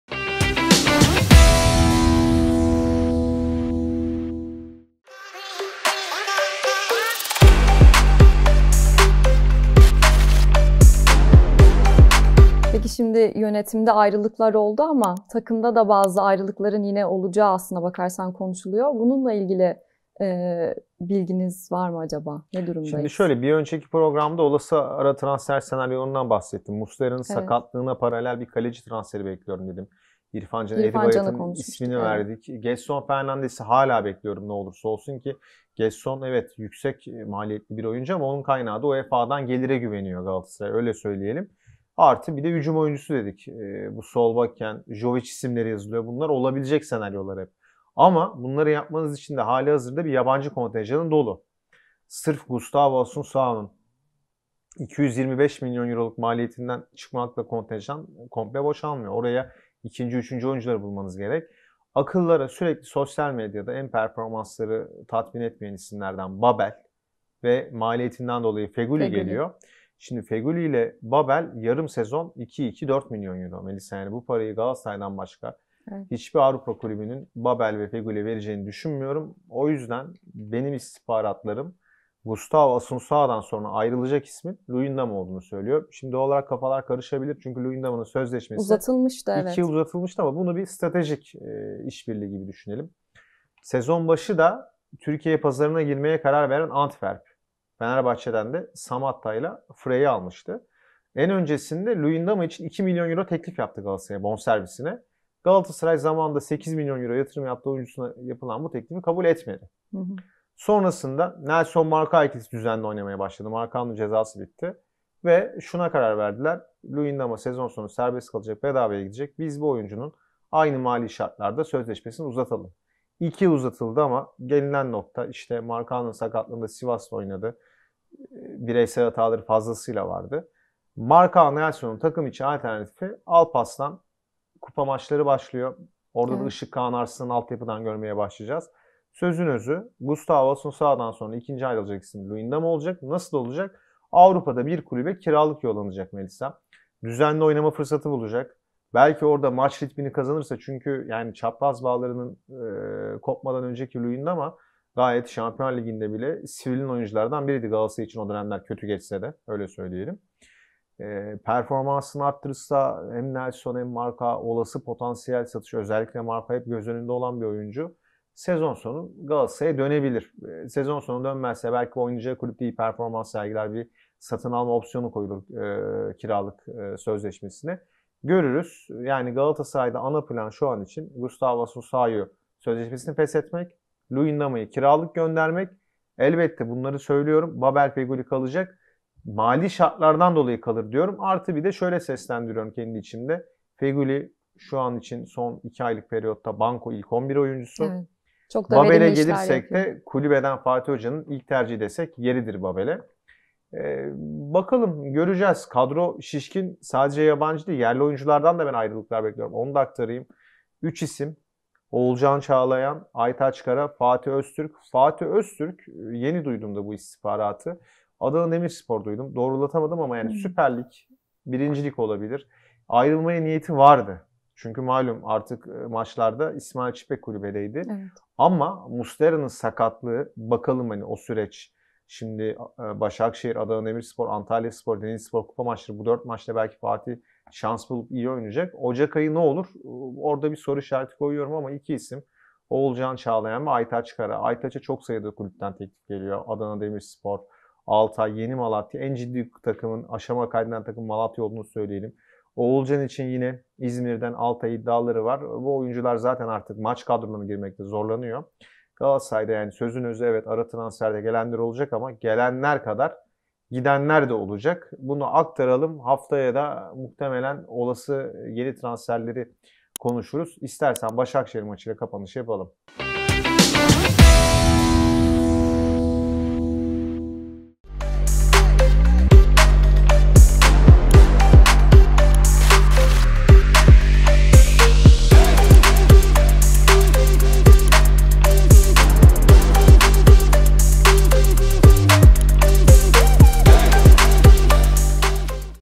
Galatasaray'da yaprak dökümü! 5 ayrılık birden
UEFA Avrupa Ligi'nde gruptan namağlup lider çıkmasına rağmen Süper Lig'de tam tersi bir görüntü çizen Galatasaray'da kadroya neşter vuruluyor. Sarı-kırmızılılarda takımdan ayrılması beklenilen 5 ismi Mehmet Özcan, Melisa Berkalp'e açıkladı. İşte detaylar...